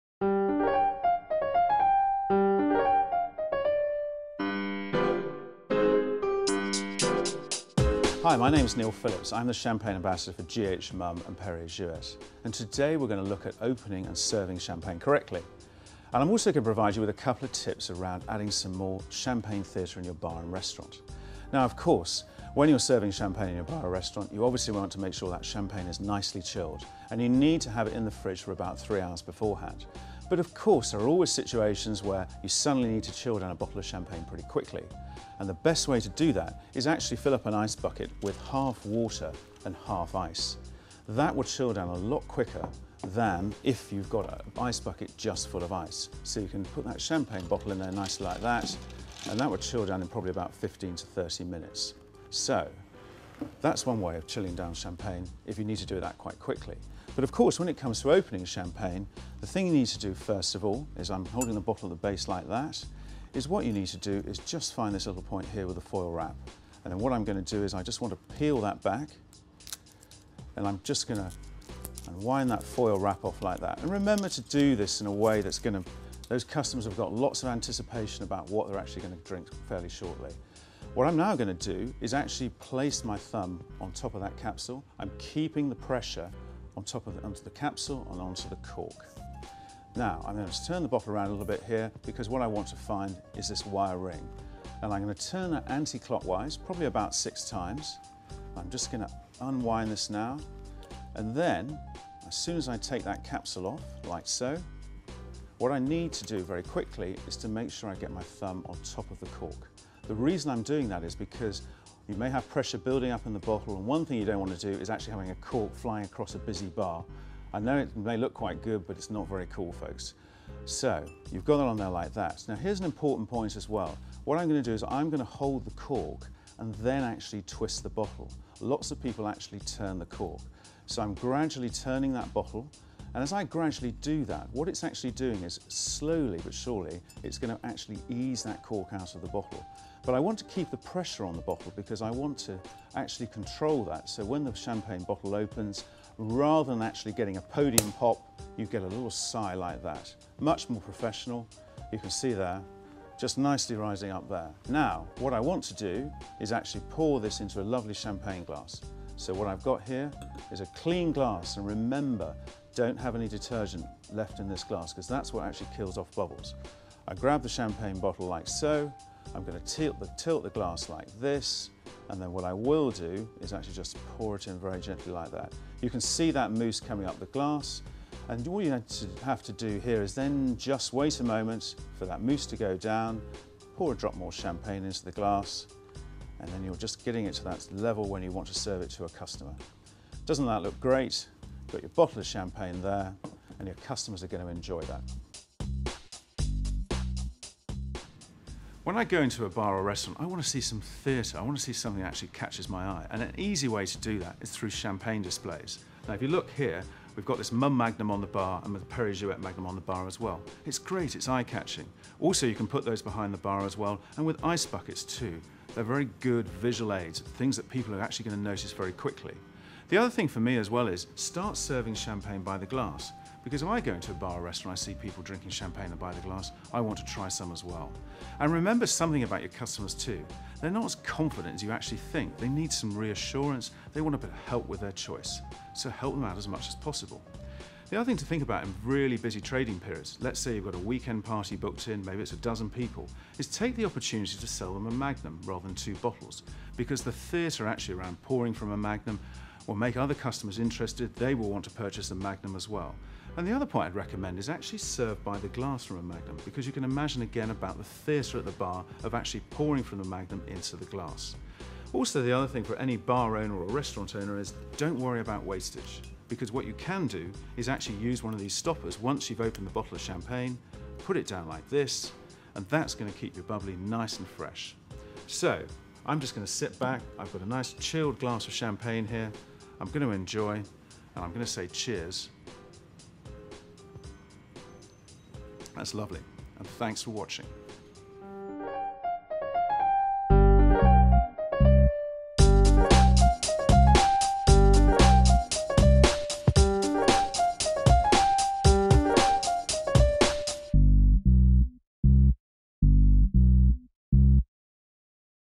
G.H.Mumm and Perrier-Jouet How to open a bottle of Champagne.
G.H. Mumm and Perrier-Jouet vodcast covering How to open a bottle of Champagne and recommendations for increasing sales by selling champagnes by the glass.